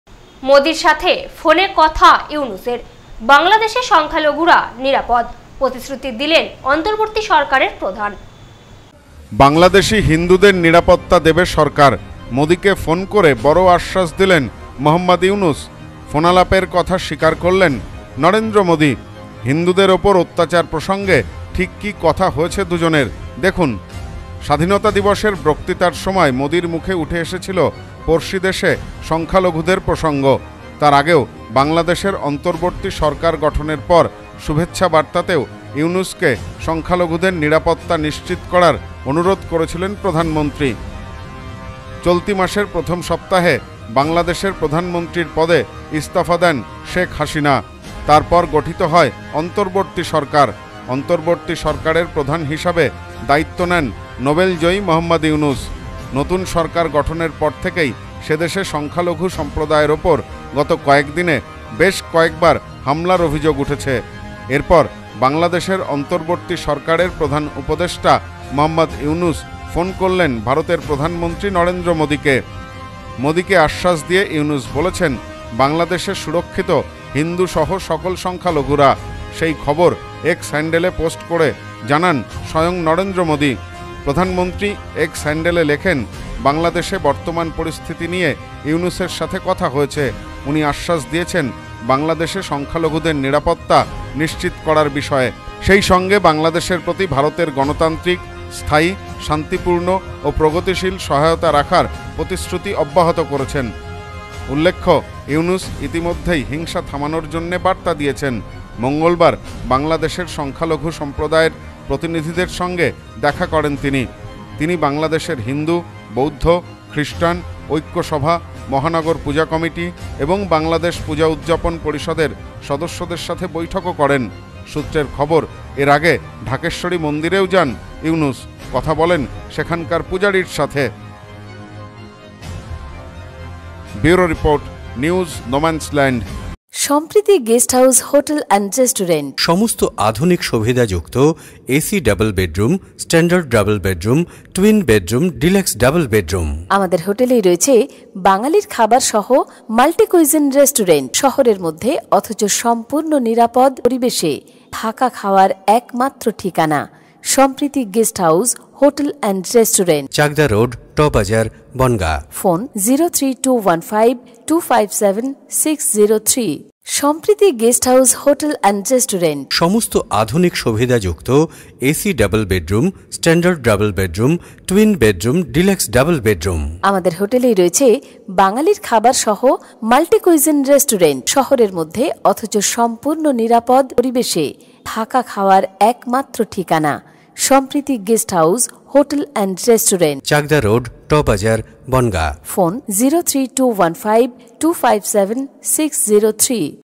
মোদীর সাথে ফোনে কথা ইউনুসের। বাংলাদেশে সংখ্যালঘুরা নিরাপদ। প্রতিশ্রুতি
মোদীর সাথে ফোনে কথা ইউনুসের। বাংলাদেশে সংখ্যালঘুরা নিরাপদ। প্রতিশ্রুতি দিলেন অন্তর্বর্তী সরকারের প্রধান।